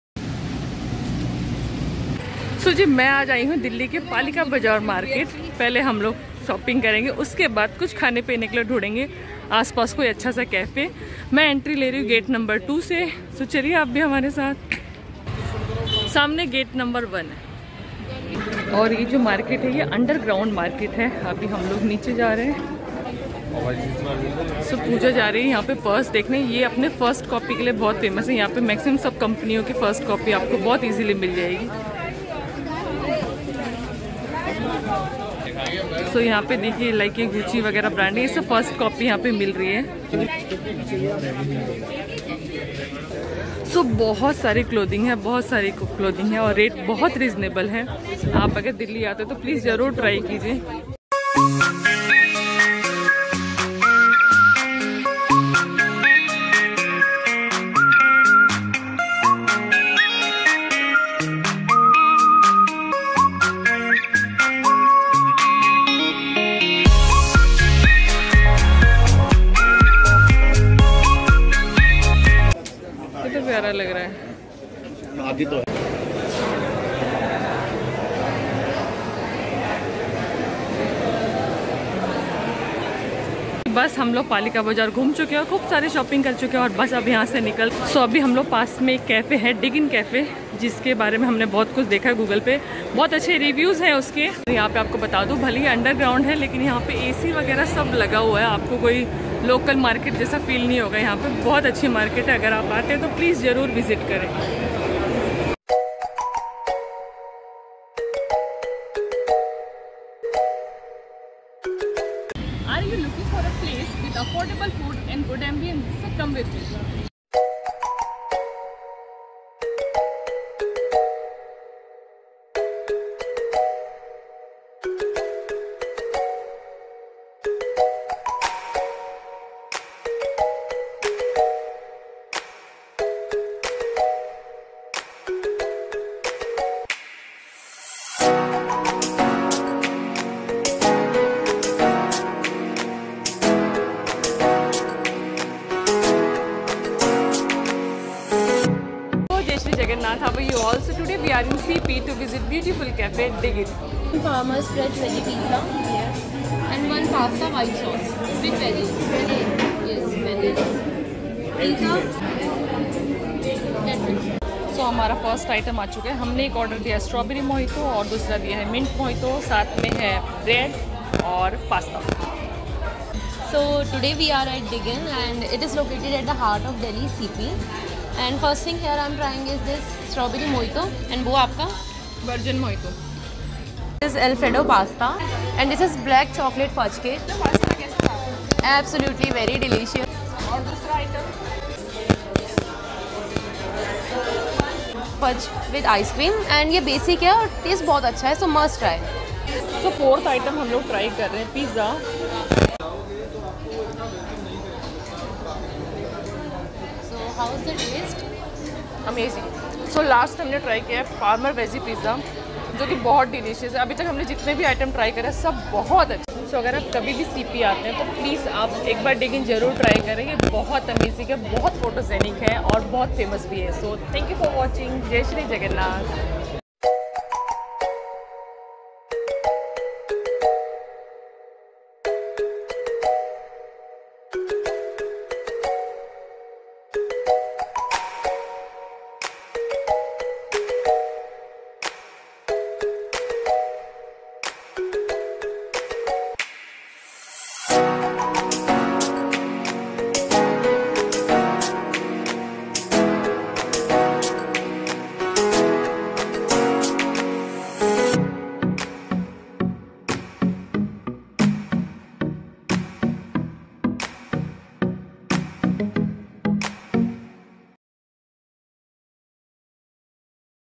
BEST CAFE FOR PHOTOGRAPHY & FOOD|| Diggin cafe#cp#palikabazar#delhifood#cafe
Best cafe for food n photography ❤️

jai shree jagannath 🙏
so i went to diggin cafe which is located in cp❤️ this place has very attractive  ambience and best for photography.  i ordered  veggie farmer's  pizza, black chocolate fudge cake, strawberry and mint mohito and alfredo pasta. All these items are mouth watering and have great presentation.

place- Diggin cafe
location- 12-a, Block A , Connaught  place, New Delhi 110053

Thankyou everyone for giving your precious  time on my video and for supporting  me ❤️
keep showering  your love❤️🙏
❤️Jai Shree Jagannath ❤️